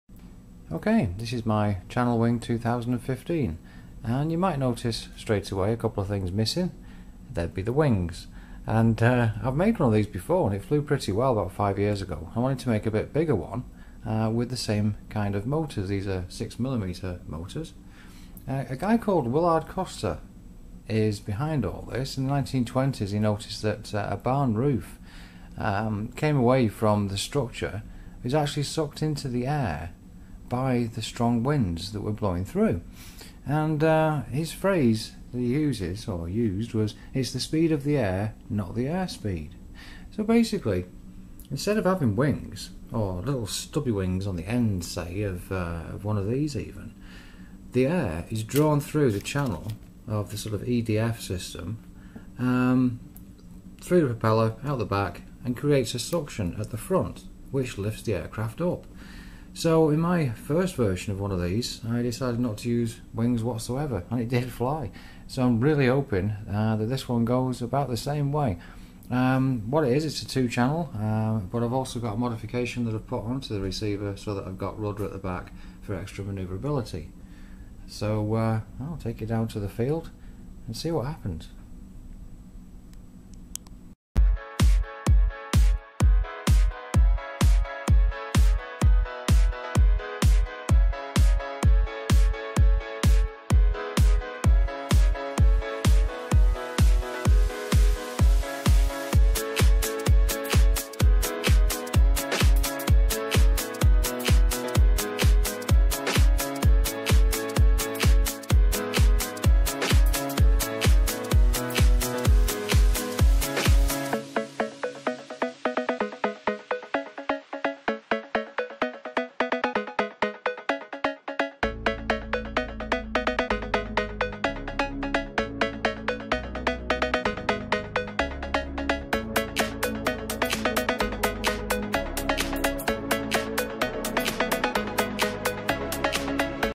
Channel Wing 2015 - No Wings !
Based on the early 20th century work of Willard Custer, this is my Channel Wing. A previous version flew well some years ago..so I wanted to build one slightly bigger, using the same sort of motors.

Total weight: 1oz, 27 grams
Length: 13", 33cm
Wingspan: erm, 0"
Receiver: 2 channel Interactive Toy Concepts
Motors: Cut down ducted fans from a Radio Shack Twin Turbine, with 6mm motors
Rudder modification: 450 turns of 40AWG, neo magnet, wires connected to +Left and +Right of the motors
Lipo: 1S 150mAh